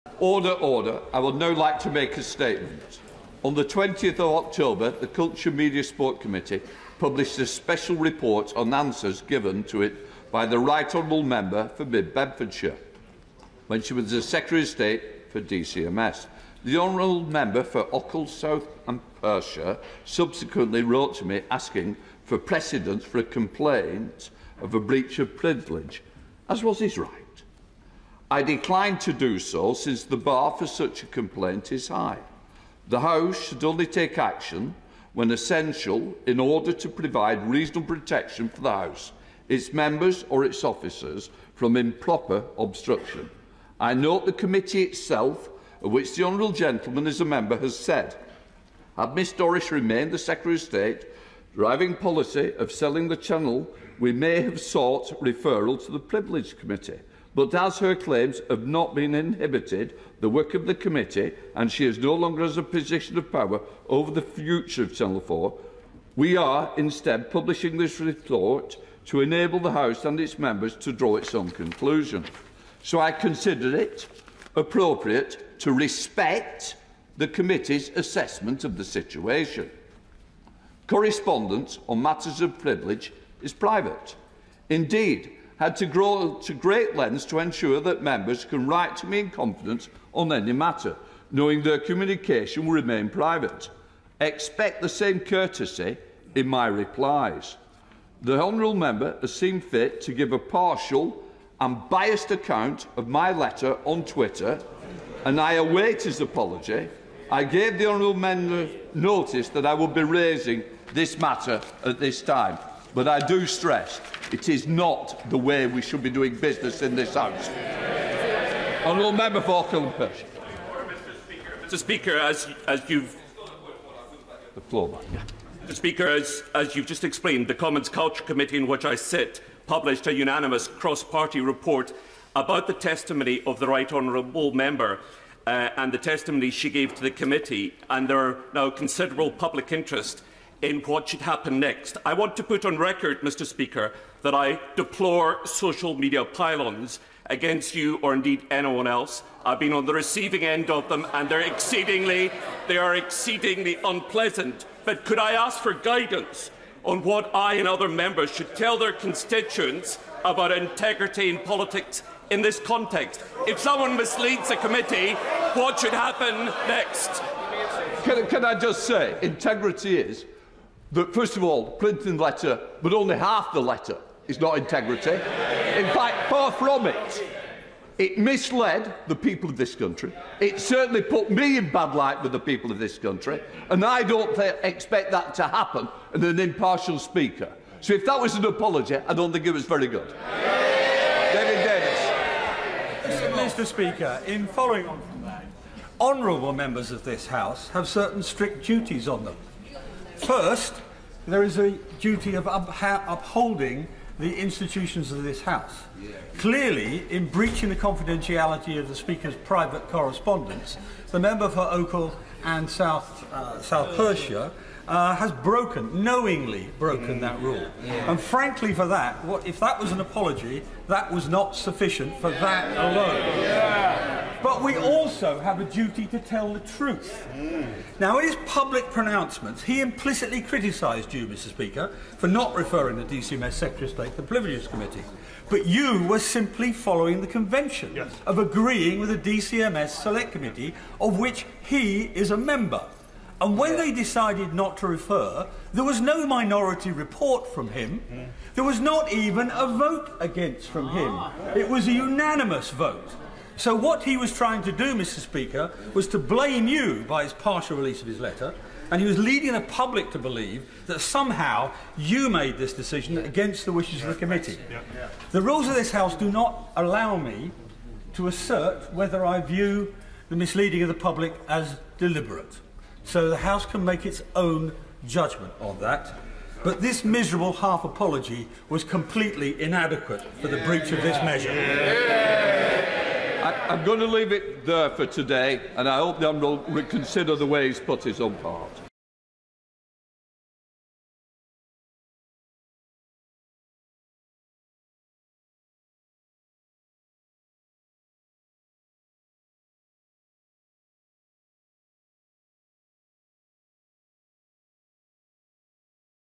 Speaker goes fully mental over Nadine Dorries lying investigation
Speaker Lindsay Hoyle made a statement calling for an apology from SNP MP John Nicolson after he posted a video on Twitter questioning why the speaker chose not to suspend Nadine Dorries from the House for misleading a Select Committee.